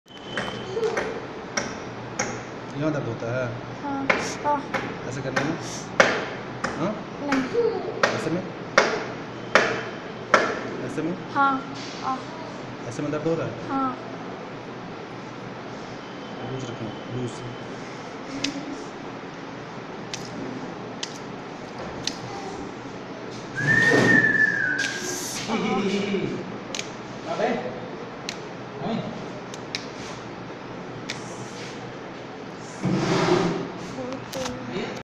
Joints creaking and Chiropractic of wrist pain Dr. Rajneesh Kant 9308511357 in india
New Release: "Back Pain Treatment || Pain Management || Dr Rajneesh Kant"
Chiropractic and manipulation